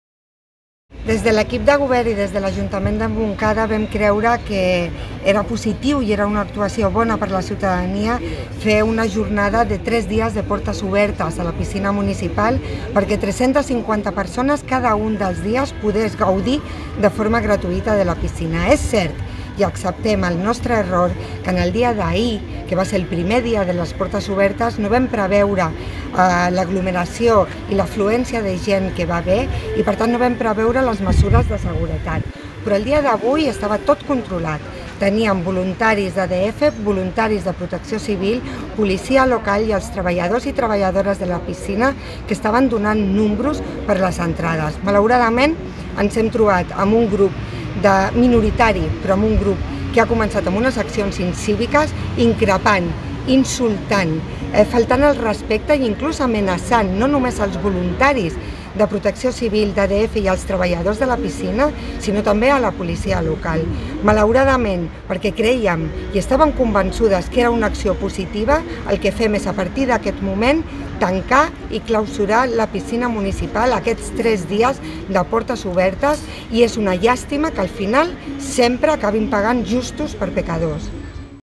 Suspensió de les jornades de portes obertes de la piscina d'estiu | Ajuntament Montcada i Reixac
L'Ajuntament ha hagut de suspendre aquesta tarda l'activitat de portes obertes a la piscina municipal d'estiu de la Zona Esportiva Centre (Bonavista, s/n) a causa de l’actitud incívica d’un grup d’usuaris que no ha respectat les mesures de control d&#39;accés i les sanitàries previstes per millorar la gestió i seguretat de la cua d’entrada al recinte.

A més, el mateix col·lectiu ha insultat i amenaçat al conjunt de treballadors municipals de l’equipament esportiu, agents de la Policia Local i voluntaris de Protecció Civil i ADF, que intentaven fer la seva feina per tal de gestionar la cua i l’accés a la piscina.